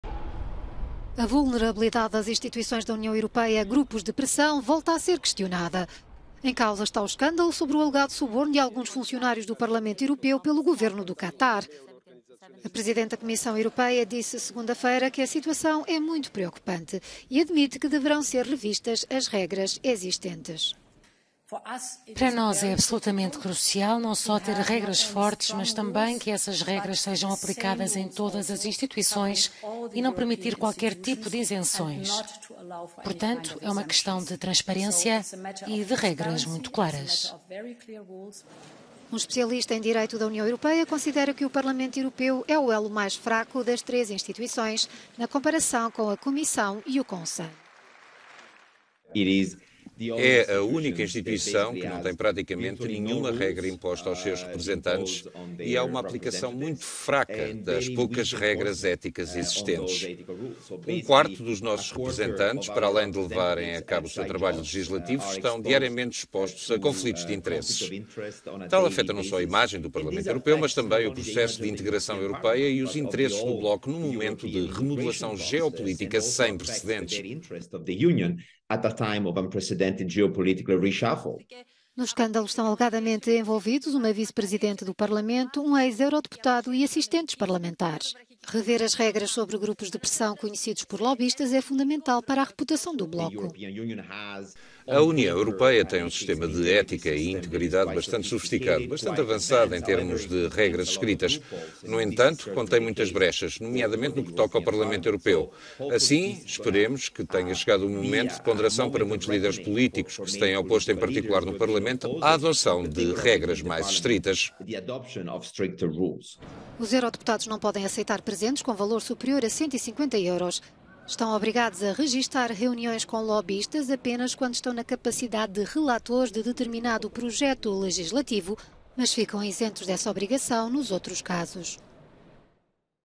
Escândalo de corrupção levará à revisão de regras para eurodeputados?
Um especialista em direito da UE considera que o Parlamento Europeu é o elo mais fraco das três instituições, na comparação com a Comissão e o Conselho.